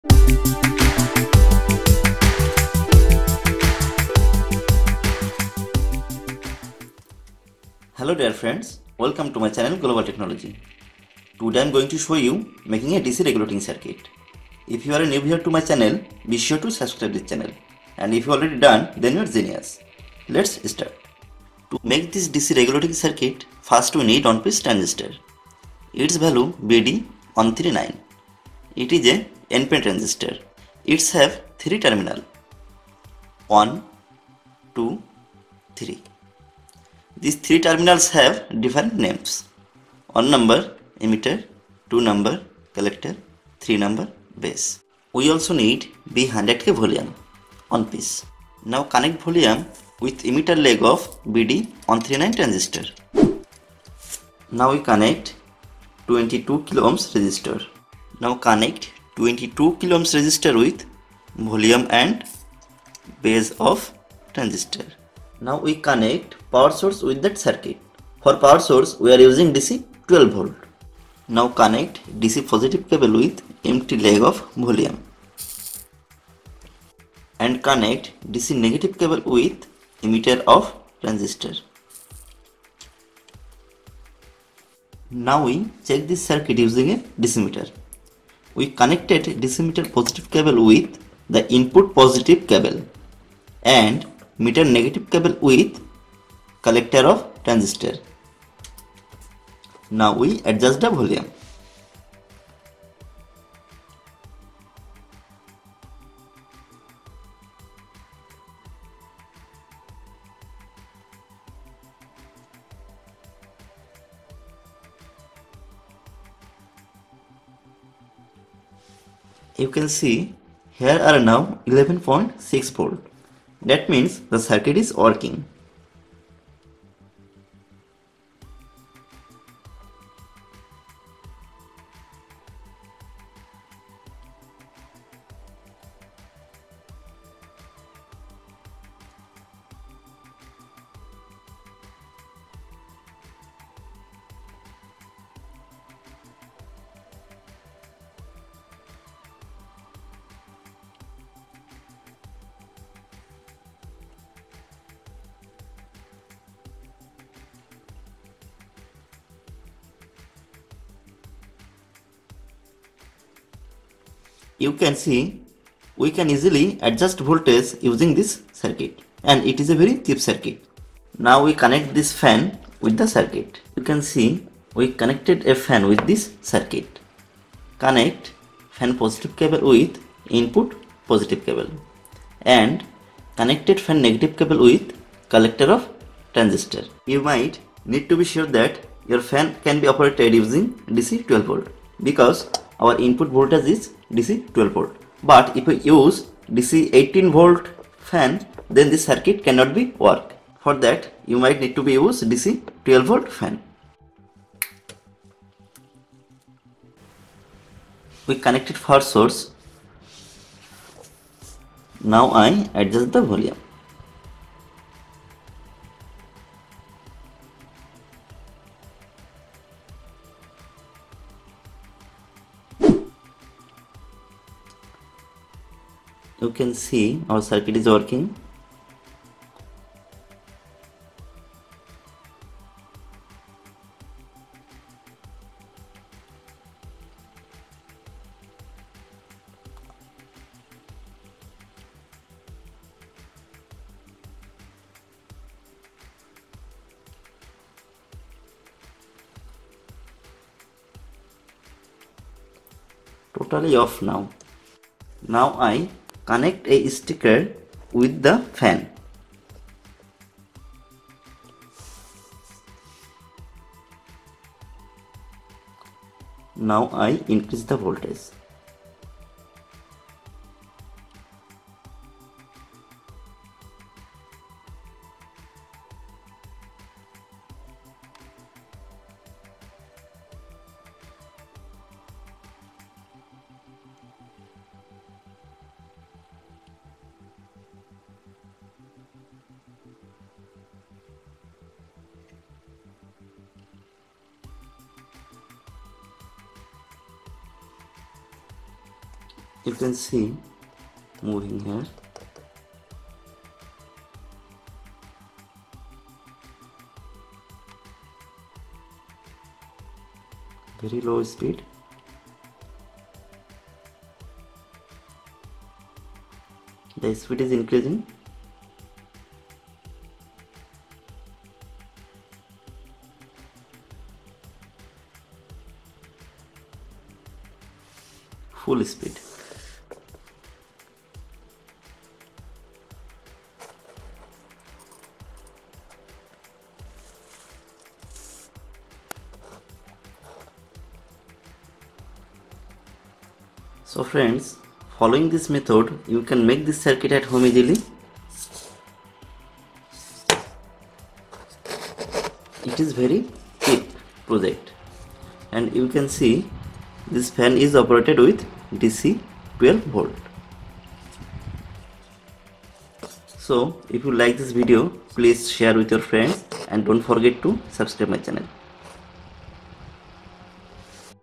How To Make Fan Speed Controller Circuit - Cheap Circuit || Full English Tutorial
In this video, we are going to show you making a simple DC fan speed controller circuit at home easily. It is a very cheap & useful electronics circuit.

To make this amplifier circuit, we might need some electronics component. Those component list have to been given below.

Electronics Component List :
1. Transistor - BD139
2. Volume - B 100 K
3. Resistor - 22 K Ohm
4. Power Source - DC 12v
5. DC Fan - 12v